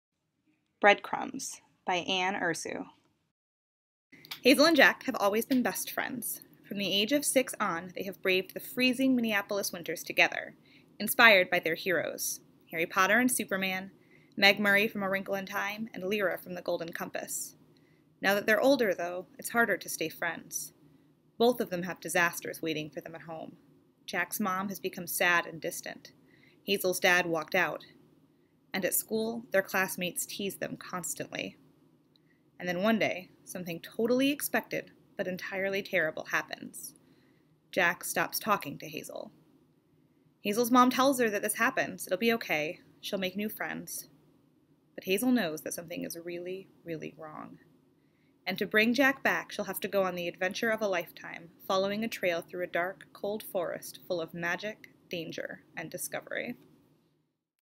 Book of the Week: Breadcrumbs
One-minute booktalk for Breadcrumbs, by Anne Ursu